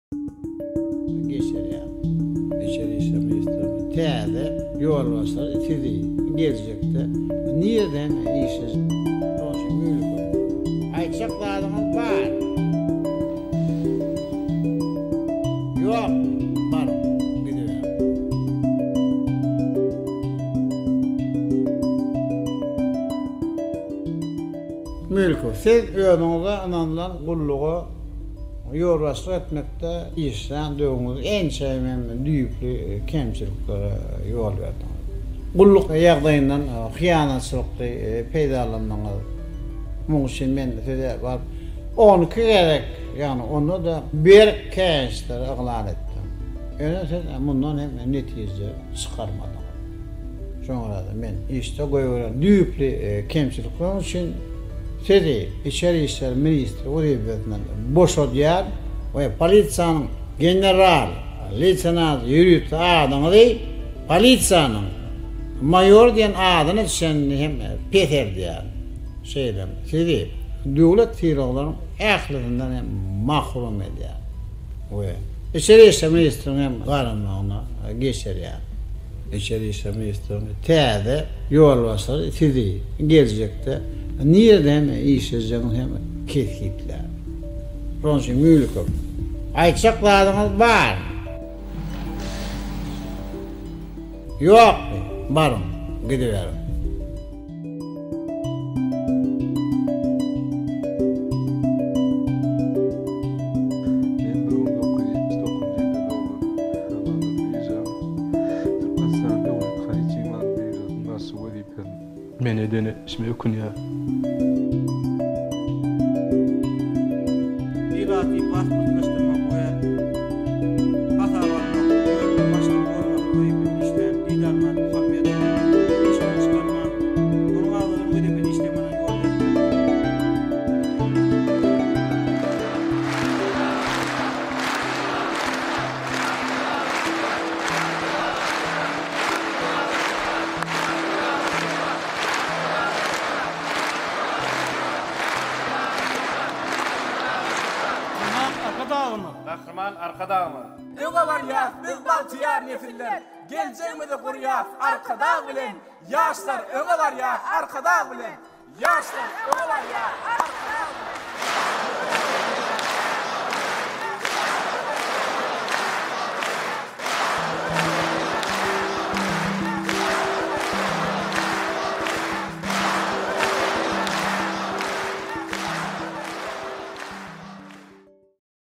'You're Fired': Turkmen President Dumps Interior Minister On TV
Turkemnistan's President Gurbanguly Berdymukhammedov has a penchant for dramatic television events. On October 1, he fired his interior minister in a humiliating spectacle.